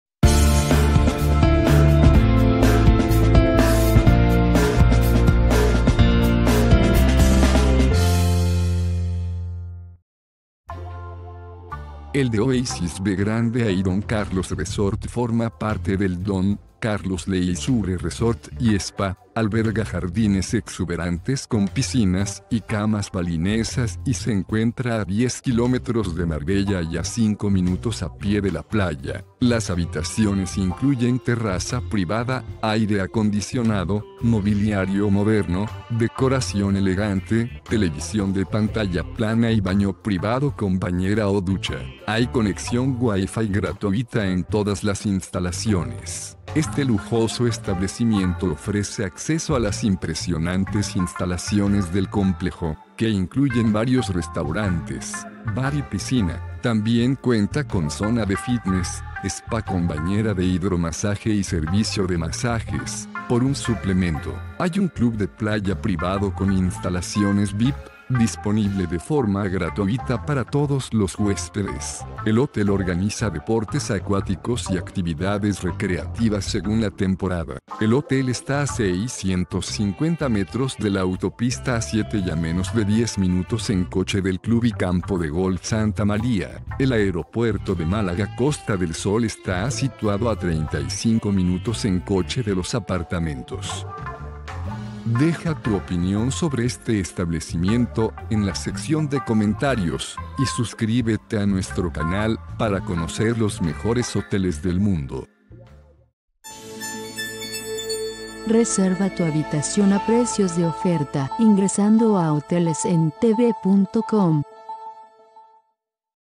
The Oasis by Don Carlos Resort en Marbella
The Oasis by Don Carlos Resort ofrece hospedaje 5 estrellas en Marbella. El inmueble se sitúa en Avenida Zurita s/n y cuenta con 35 habitaciones con minibar, ducha y bañera.